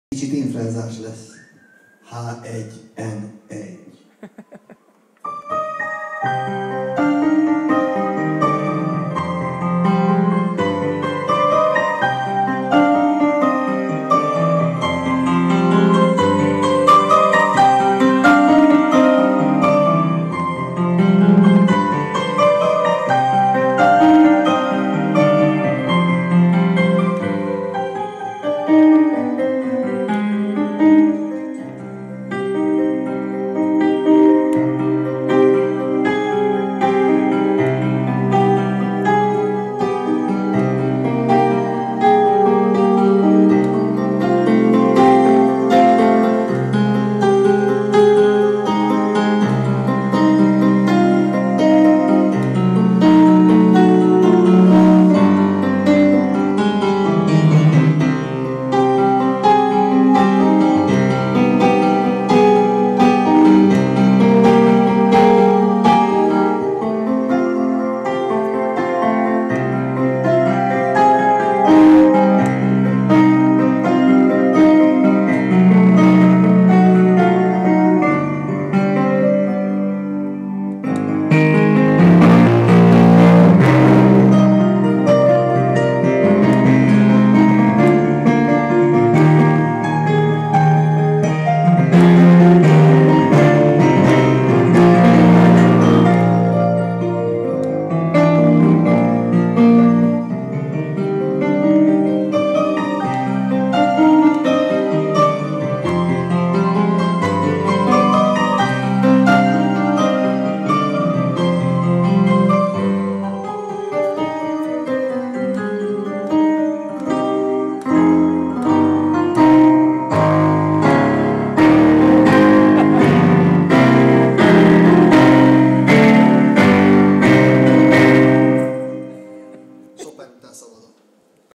Edda Művek, Gömöry Zsolt a próbán.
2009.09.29. A 2009. október 10-i PECSA koncert próbáján.